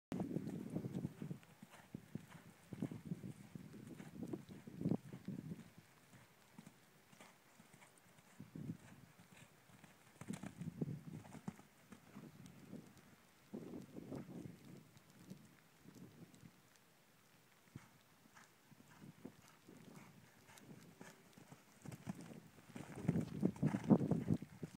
Carousel Jazz
For Sale - 11yr old mare. 16.hh. Won £750 British show jumping. Bred a foal. Video taken shortly after being back in work.